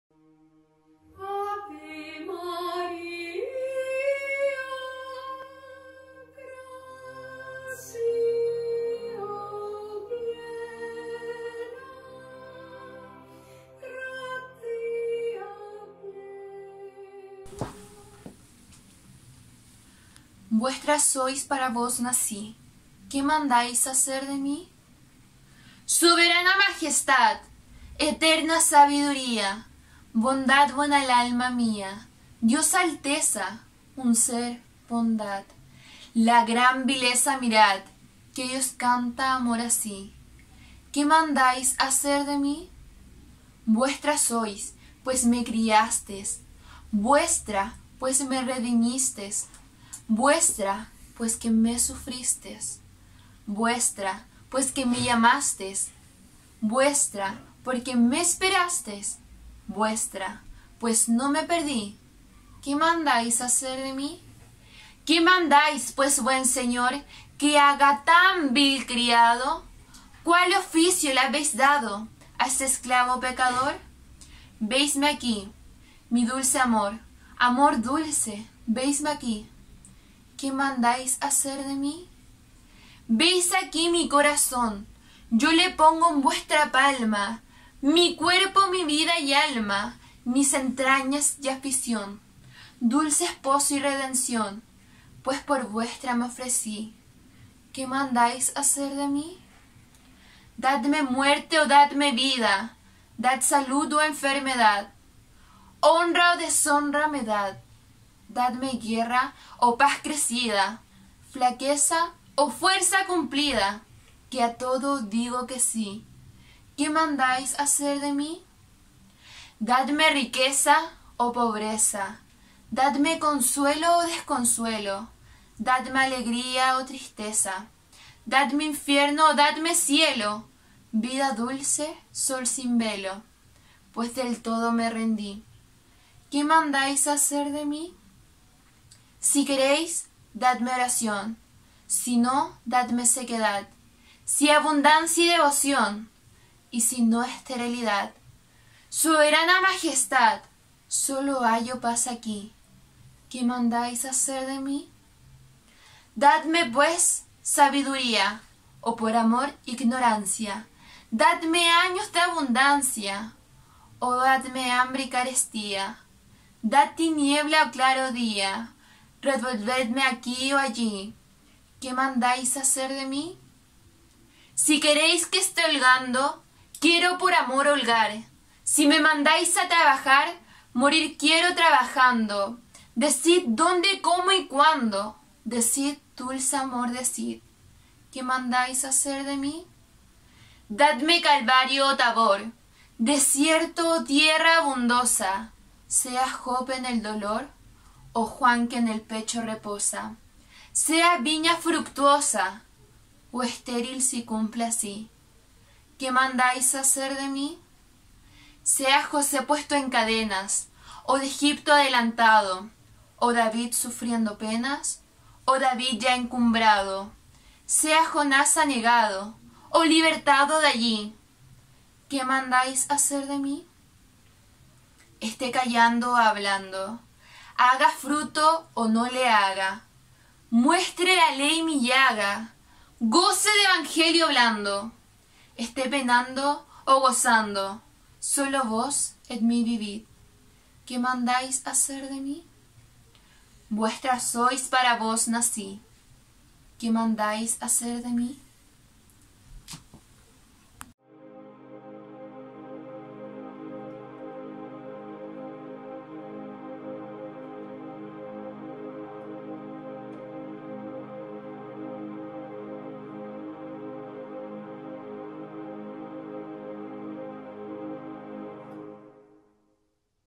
Compañía Beatus Ille
IV Festival de Poesía y Teatro Clásico en línea «Pedro Calderón de la Barca»

«Vuestra soy, para vos nací» de Santa Teresa de Jesús.
Recitantes: Thais Candia.
Dirección: Camila Torres.
Cerrillos - Santiago de Chile.